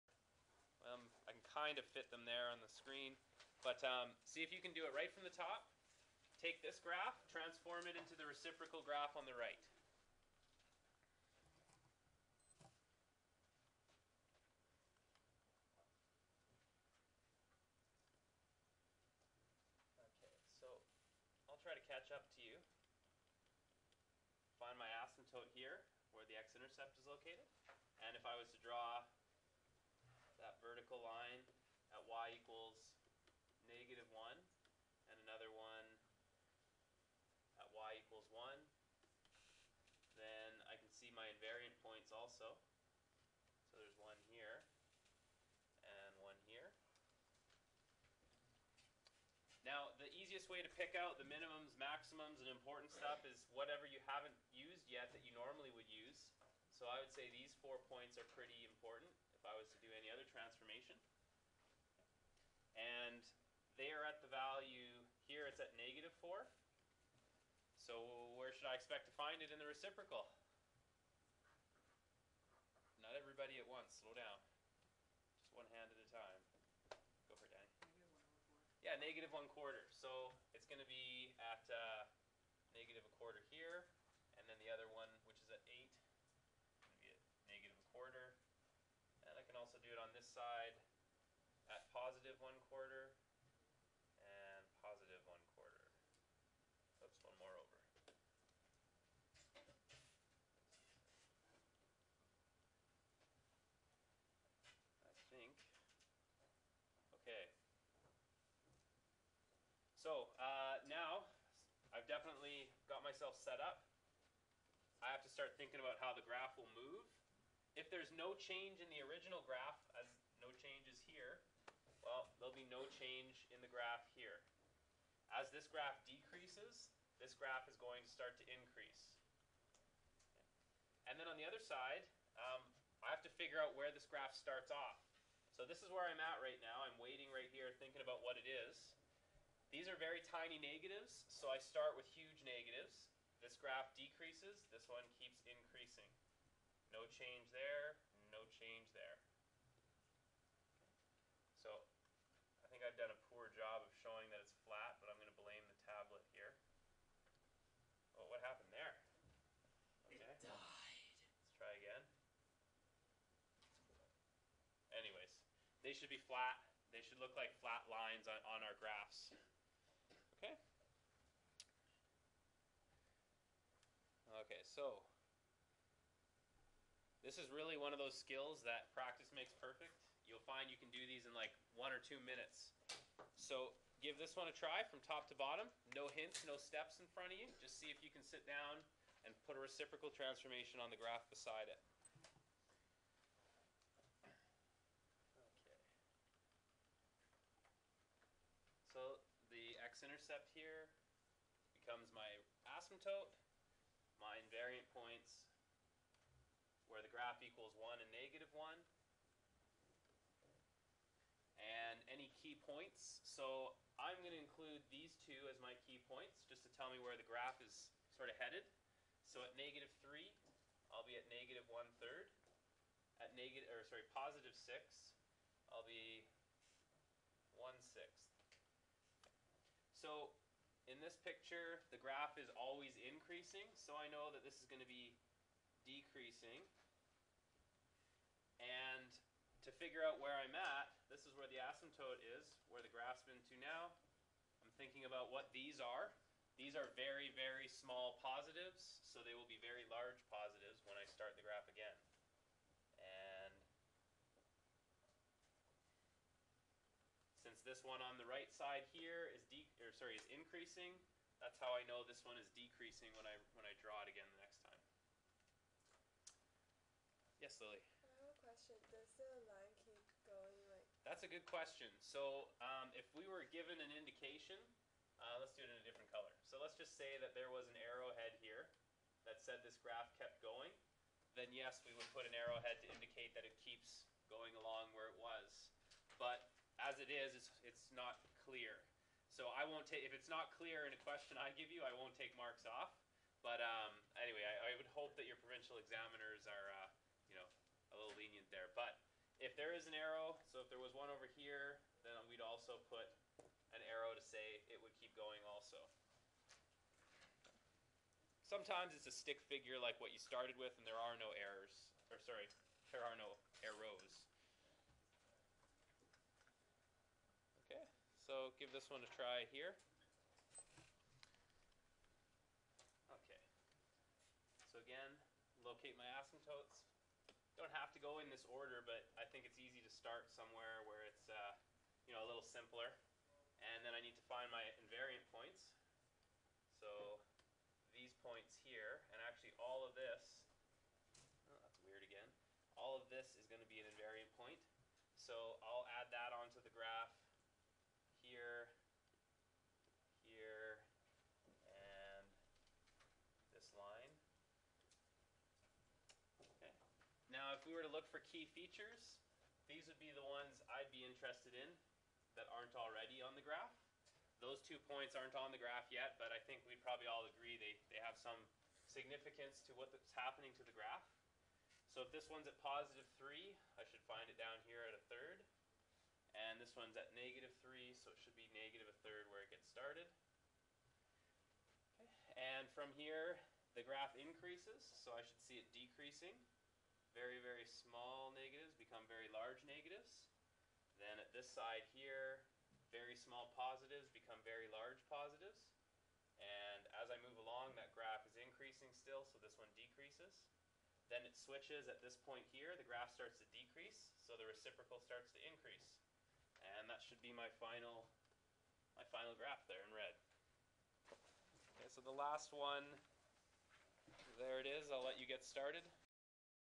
Mr Joyce Math 12 - Reciprocal Transformations III
In the last lesson of our transformations unit we look at how to perform a reciprocal transformation.